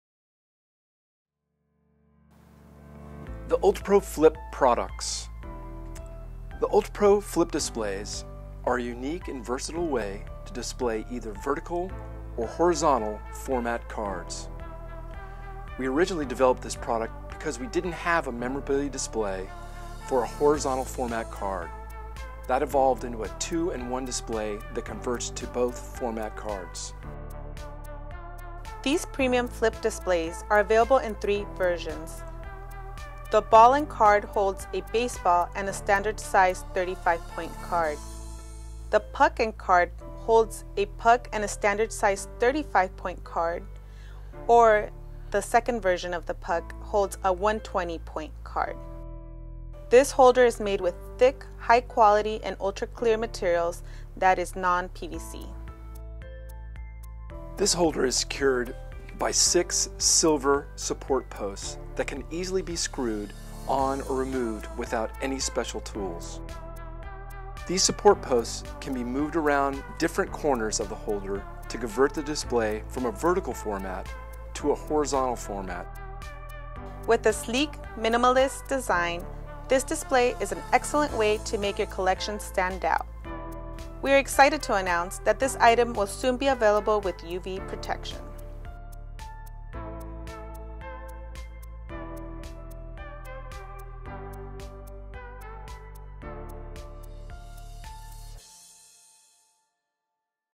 Flip Display (Puck & Baseball)
The newest and most versatile display for baseball or hockey fans!
Put your most valuable player on display with Ultra PRO's clear Flip Display. This holder is made with thick, high quality and ultra-clear materials that is non-PVC. The holder is secured by six (6) silver support posts that can be moved around different corners of the holder to convert the display from a vertical format into a horizontal format to accommodate trading cards that are printed horizontally. With a sleek, minimalist design, this display is an excellent way to make your collection stand out.